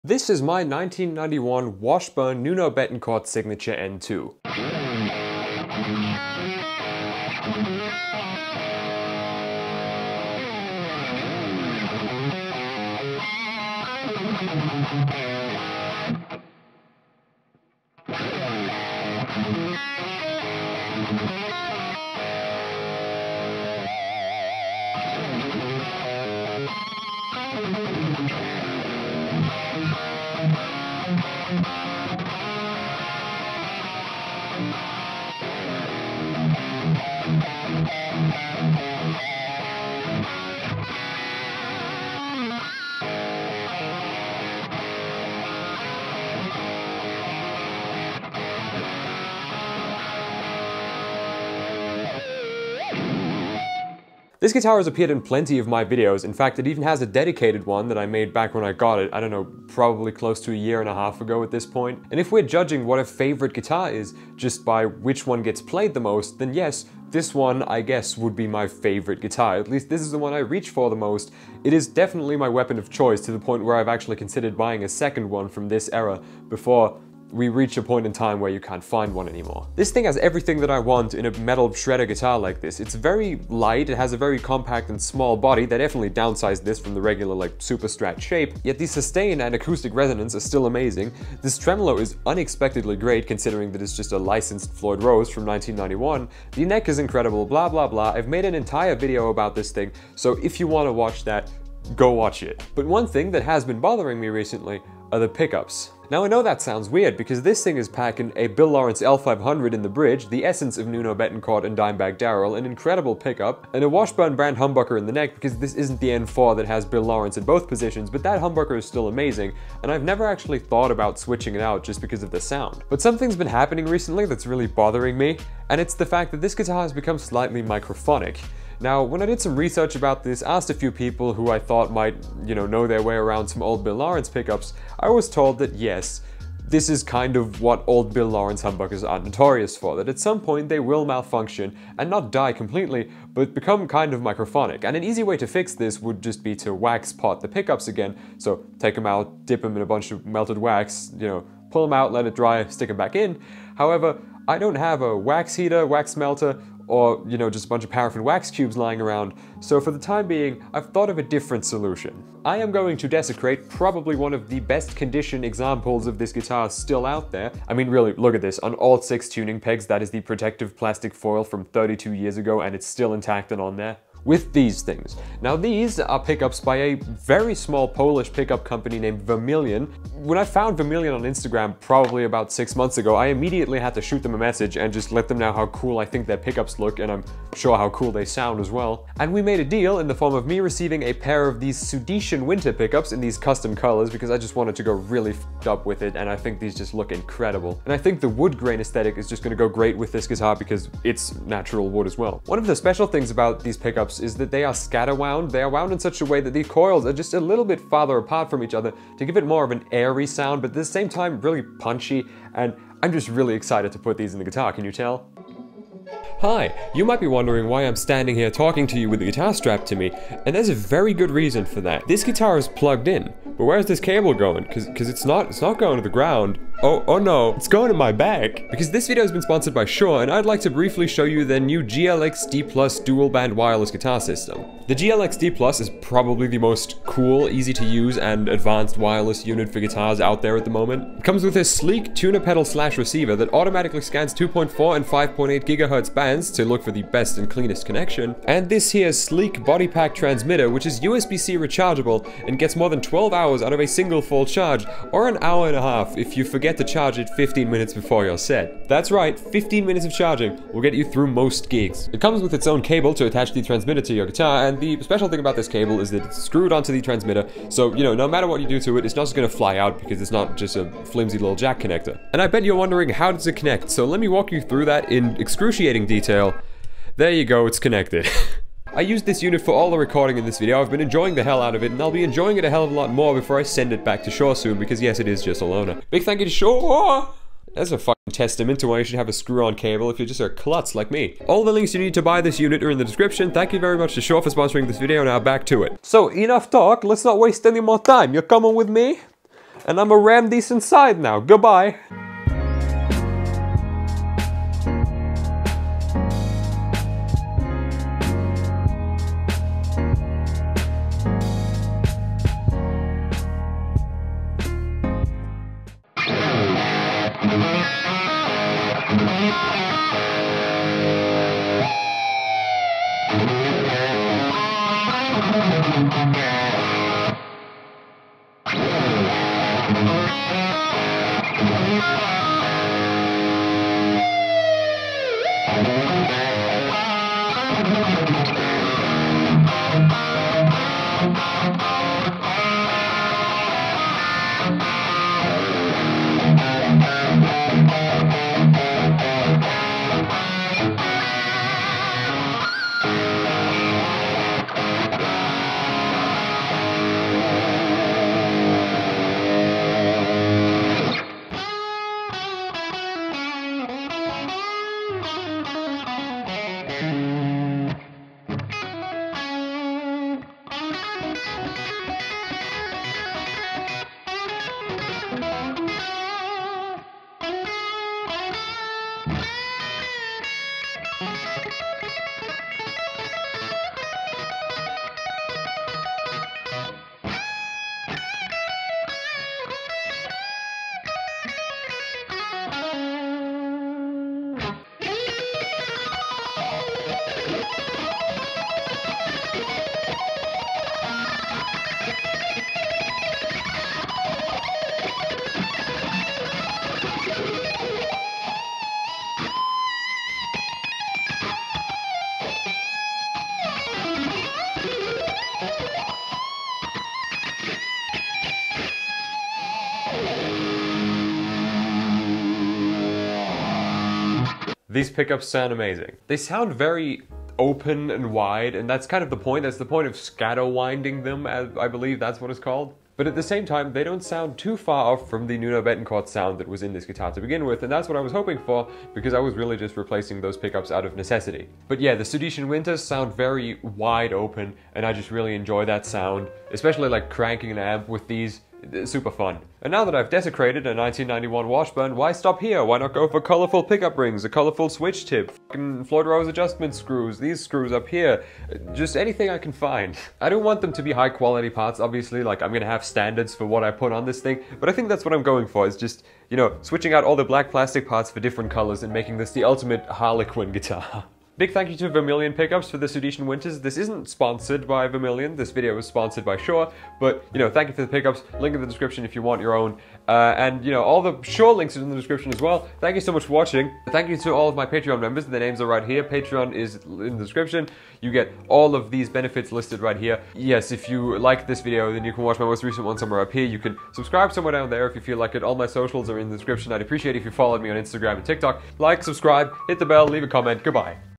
Modifying a RARE Washburn N2
I'm looking forward to modding this guitar further (any minor future mods will be posted on my Instagram)